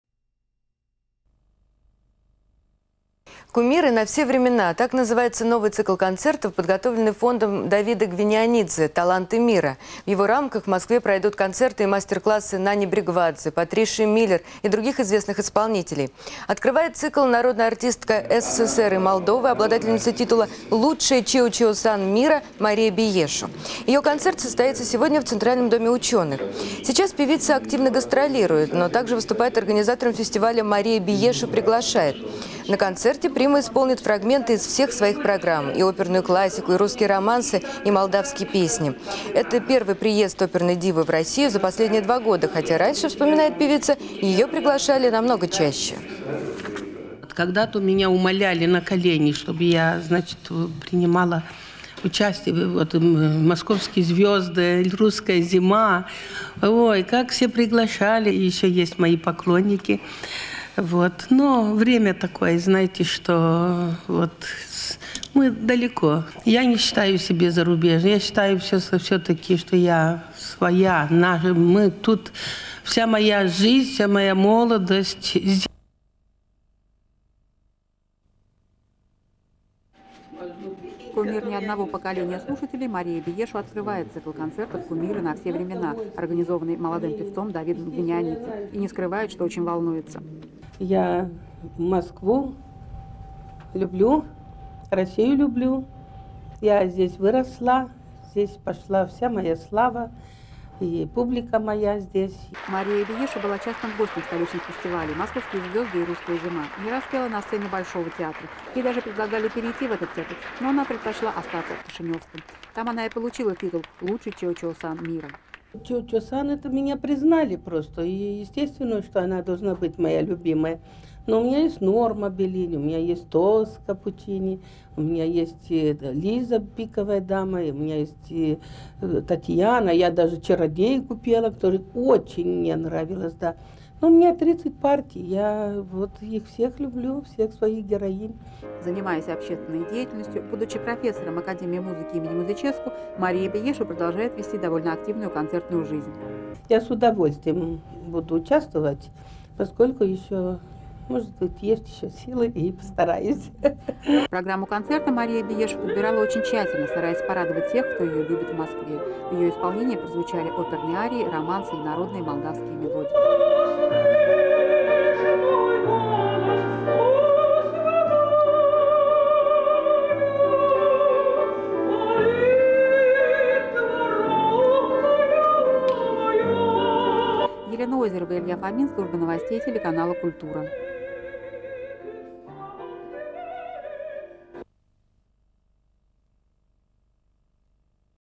David Gvinianidze & Maria Bieshu Moscow 2006
Концерт фонда "Таланты мира" из цикла "Кумиры на все времена" с участием М. Биешу и Д. Гвинианидзе